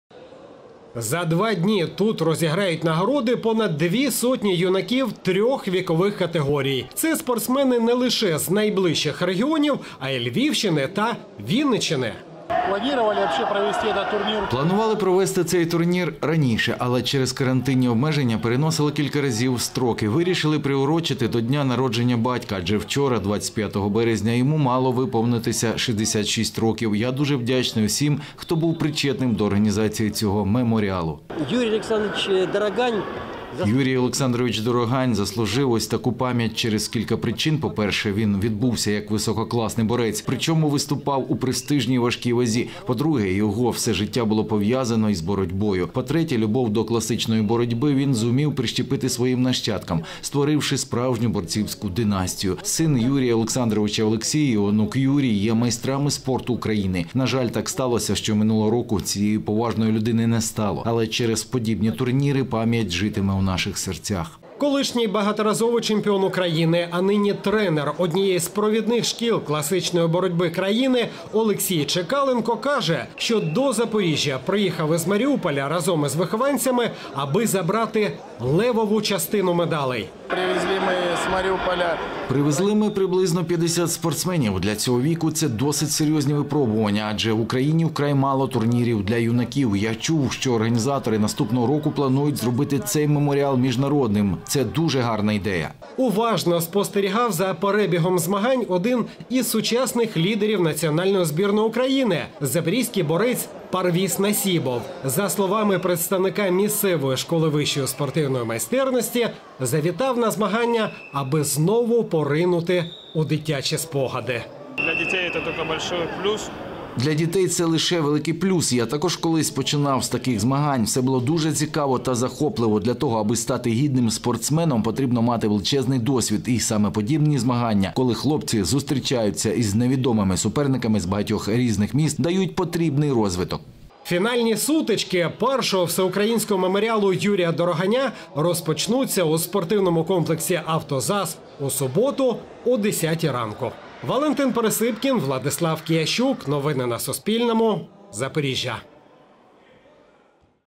Турнір з греко-римської боротьби | Новини
У Запоріжжі розпочався перший Всеукраїнський юнацький турнір з греко-римської боротьби пам'яті майстра спорту СРСР Юрія Дороганя. Дводенні змагання, що відбуваються у спортивному комплексі “АвтоЗАЗ”, зібрали під свої прапори юних представників багатьох відомих вітчизняних шкіл розвитку класичної боротьби.